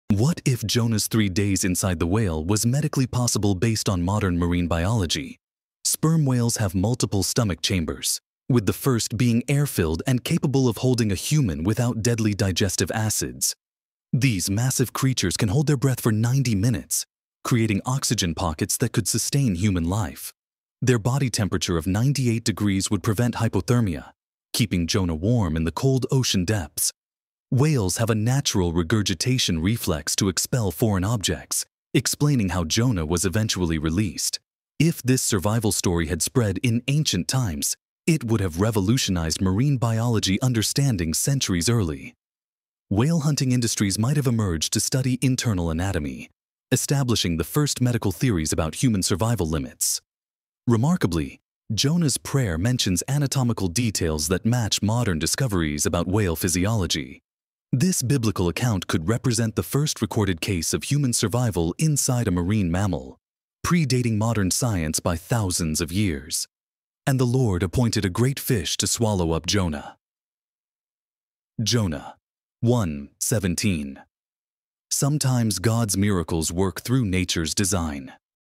What If Jonah Actually Survived Inside a Whale? Science Explains How
Jonah + whale = actually possible? Mind blown 🐋